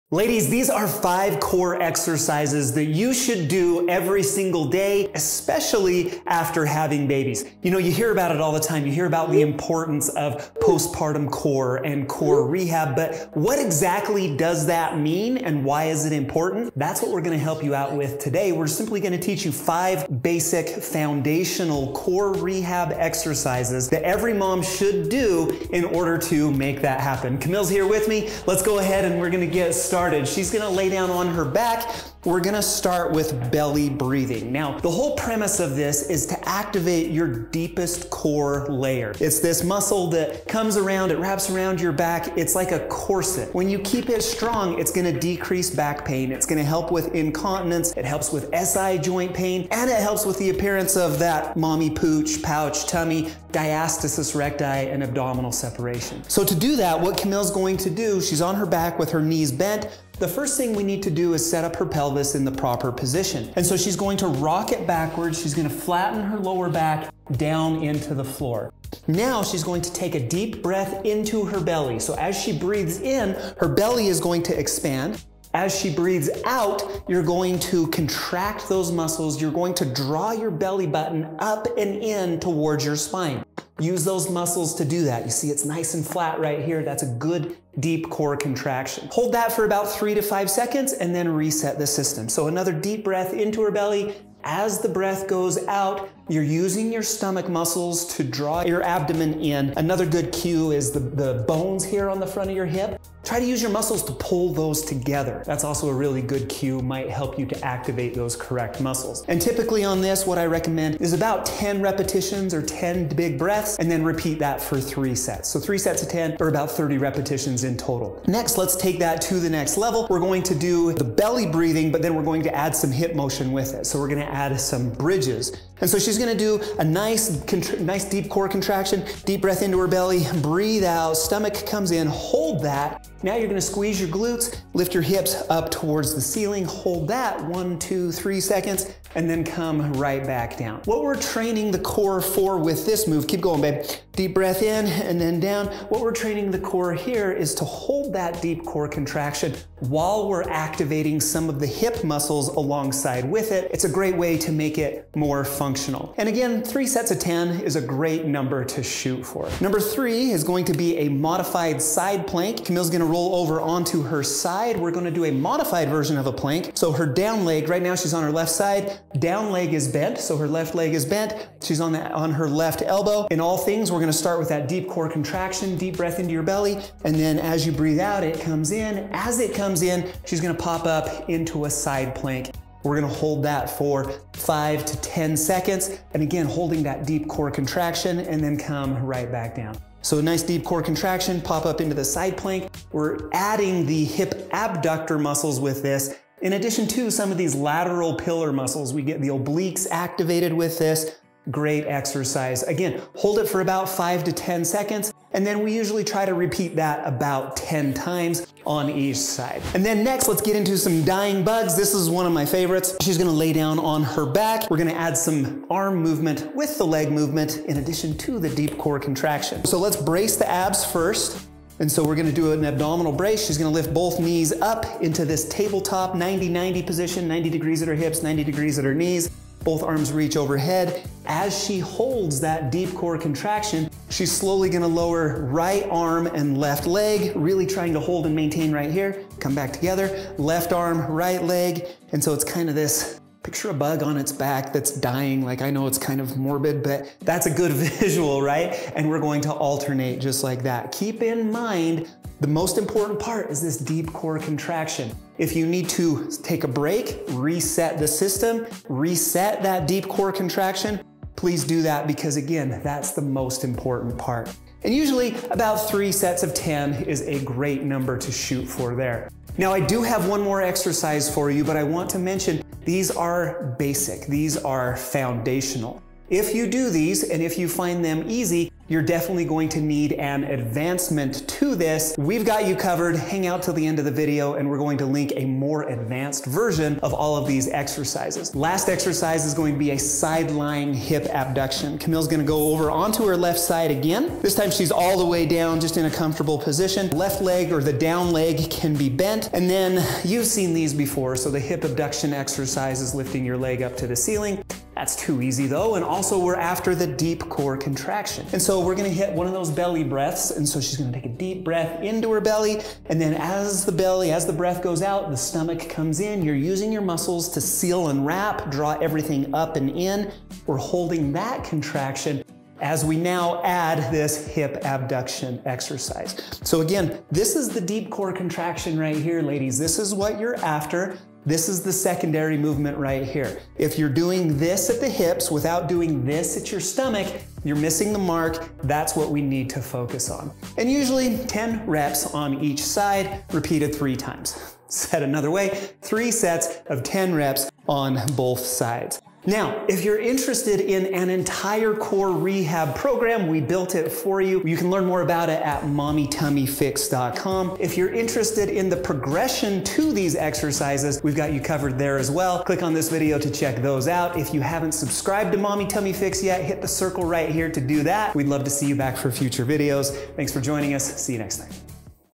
5 of the Best Core Exercises for Moms To Do Everyday
After child birth, every mama would benefit from deep core strengthening exercises (especially if she has diastasis recti). As a doctor of physical therapy, these are the 5 postpartum core exercises that I recommend.

These postpartum core moves are safe to do every single day.

Having a strong core helps relieve and prevent back pain, many pelvic floor issues, and ab separation (also known as diastasis recti). Plus these exercises will help to flatten your stomach and eliminate a "mommy tummy" that seems to linger long after you deliver your baby.

The BEST postpartum ab exercises are those that activate the transverse abdominis muscle and then progress around this contraction. This is the muscle that you activate to draw your navel in towards your spine. It's the "corset" muscle that helps stabilize your spine and trim down your waistline. The stronger this muscle the tighter it becomes - thereby shrinking your waistline and helping to eliminate "mommy tummy".

BENEFITS OF HAVING A STRONG CORE

There should be more motivation to doing core workouts than just having a 6 pack or flat belly. If you strengthen your core with regular core exercises, you will build a strong body that will benefit you for life

Pretty much any movement uses our core strength like walking, sitting, exercising, etc. If you have a strong core, it will be easier to do many activities like swinging a baseball bat to reaching up to the top shelf of the grocery store to bending down to tie your shoes.

Your core helps stabilize your entire body- it is where your center of gravity is focused. A strong core will bring better posture, less lower back pain, and more overall stability.

EXERCISES IN THIS VIDEO:
1. Posterior Pelvic Tilt (and Belly Breathing)
2. Bridge
3. Modified Side Plank
4. Dying Bug
5. Side Lying Abductions

AMAZON STORE

TIME STAMPS:
0:00 Intro
0:29 Belly Breathing and Posterior Pelvic Tilt
2:00 Bridges
2:40 Modified Side Plank
3:30 Dying Bug
4:49 Side Lying Abductions